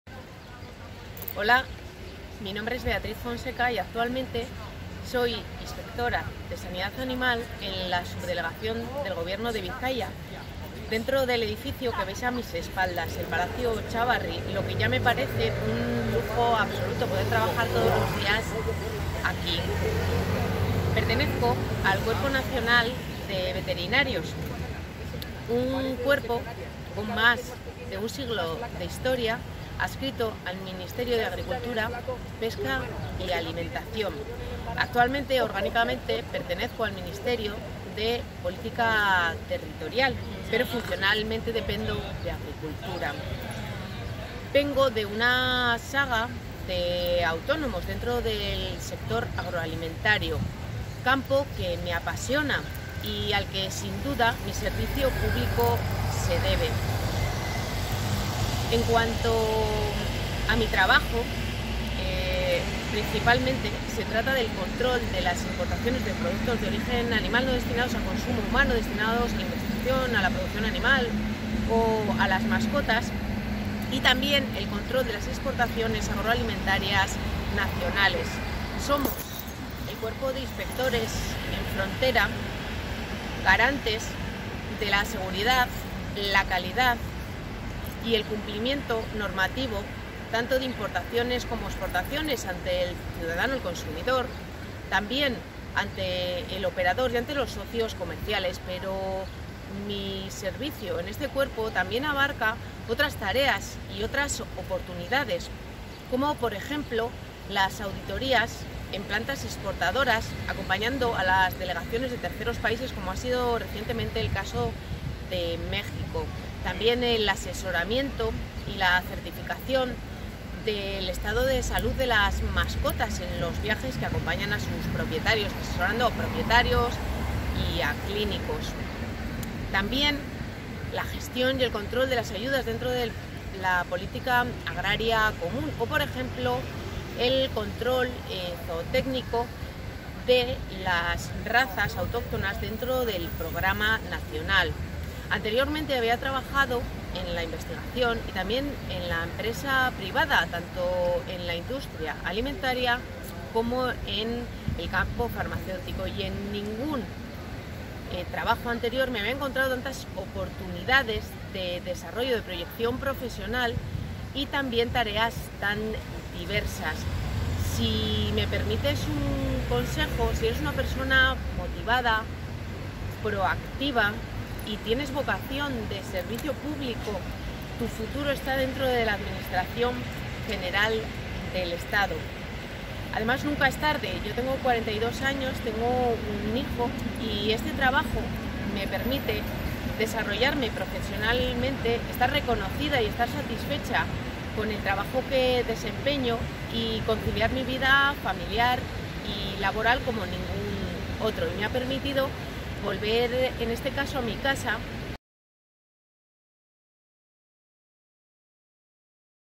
Beatriz Fonseca. Cuerpo Nacional de Veterinarios. Talento público.
Beatriz Fonseca. Cuerpo Nacional de Veterinarios, presta sus servicios dentro de la Subdelegación del Gobierno de Vizcaya como Inspectora de Sanidad Animal, adscrita al Ministerio de Agricultura, Pesca y Alimentación, aunque orgánicamente pertenece al Ministerio de Política Territorial. Son el Cuerpo de inspectores en frontera garantes de la seguridad, la calidad y el cumplimiento normativo tanto de importaciones como de exportaciones ante el ciudadano y el consumidor. Y como consejo nos dice que si eres una persona motivada, proactiva y tienes vocación de servicio público, tu futuro está dentro de la Administración General del Estado.
Más información sobre empleo público